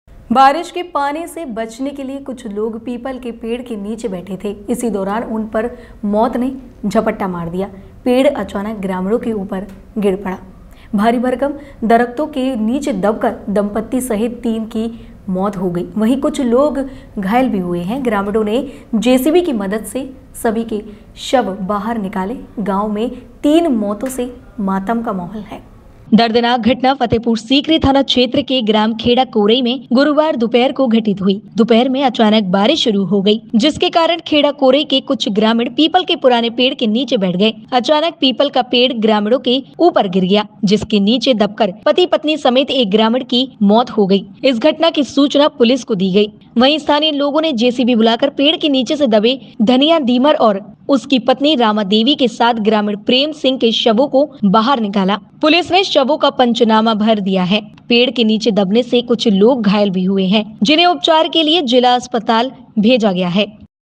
थाना फतेहपुर सीकरी क्षेत्र के गांव खेड़ा कोरई की घटना
बारिश के पानी से बचने के लिए कुछ लोग पीपल के पेड़ के नीचे बैठे थे। इसी दौरान उन पर मौत ने झपट्टा मार दिया। पेड़ अचानक ग्रामीणों के ऊपर गिर पड़ा। भारी भरकम दरख्तों के नीचे दबकर दंपती सहित तीन की मौत हो गई। वहीं कुछ लोग घायल भी हुए हैं। ग्रामीणों ने जेसीबी की मदद से सभी के शव बाहर निकाले। गांव में तीन मौतों से मातम का माहौल है।

praatahdarpannews